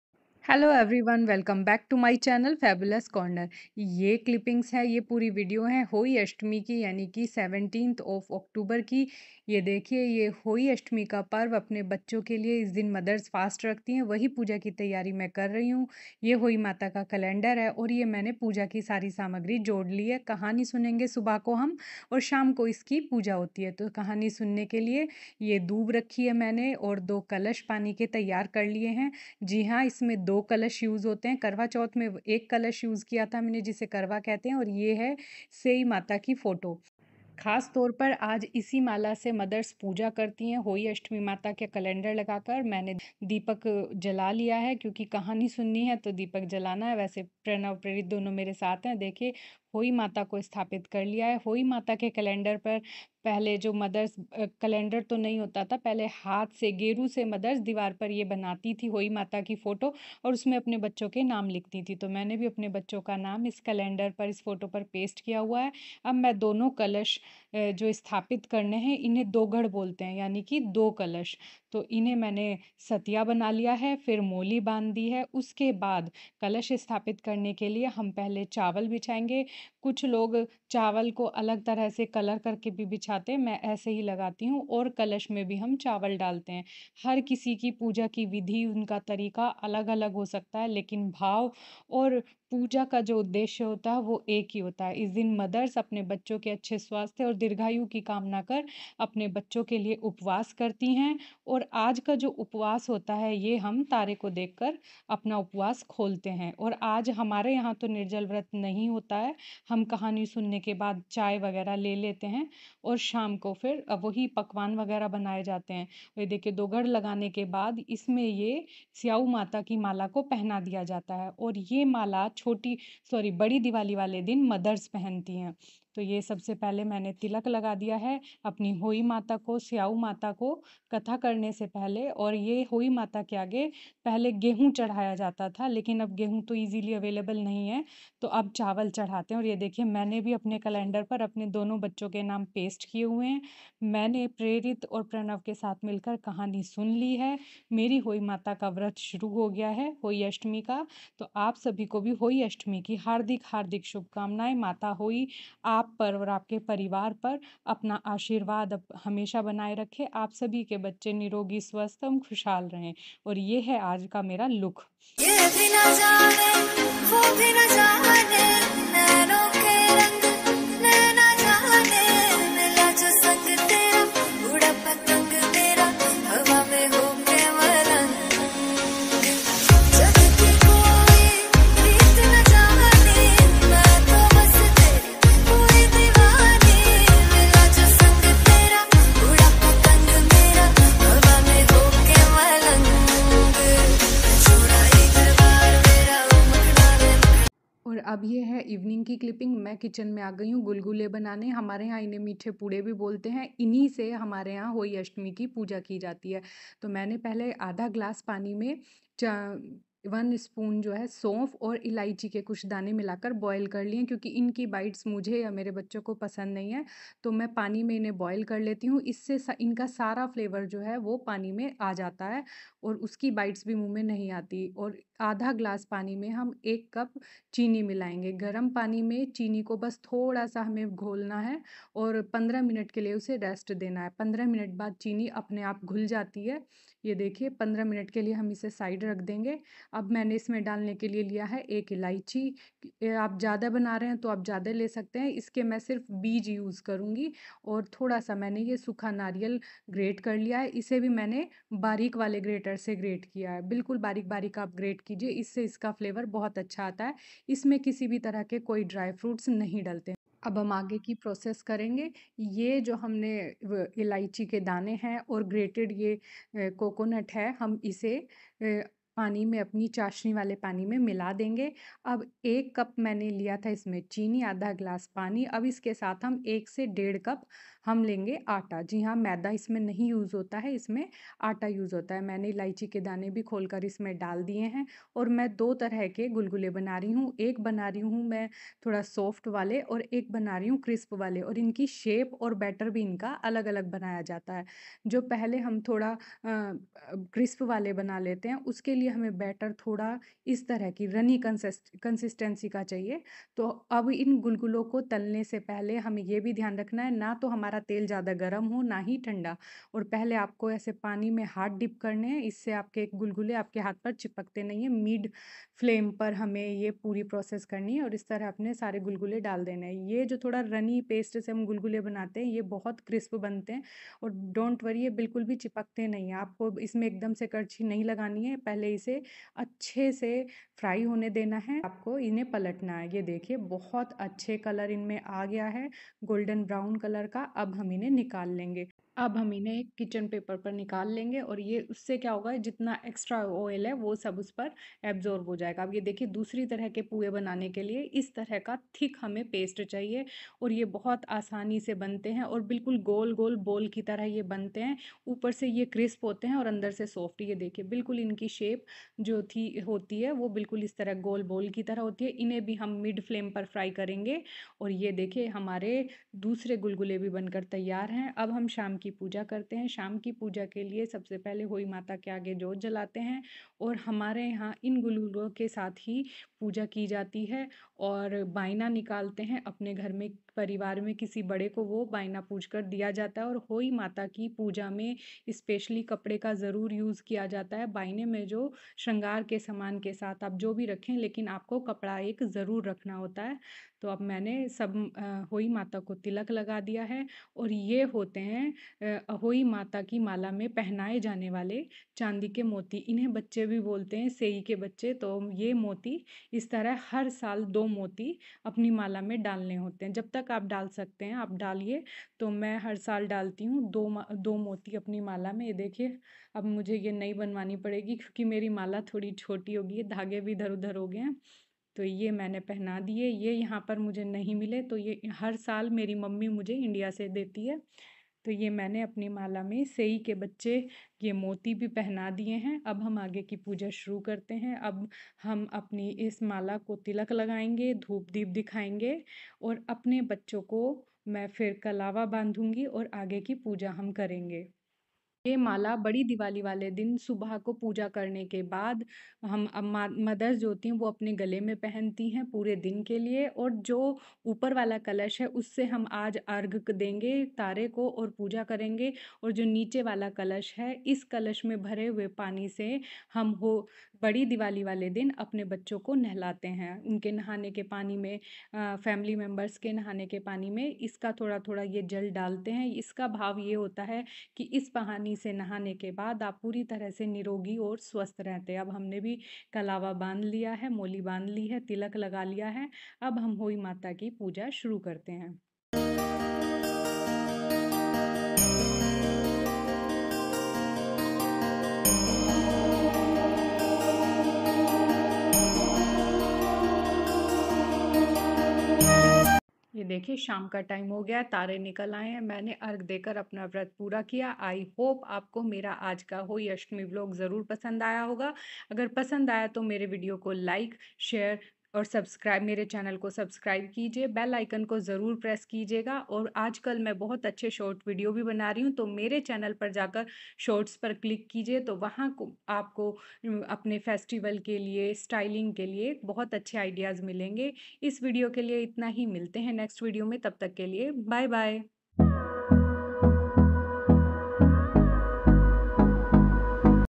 Ahoi Ashtami Festival celebration/2 different Gulgule recipes/styling my wedding saree
Music is taken from channel:-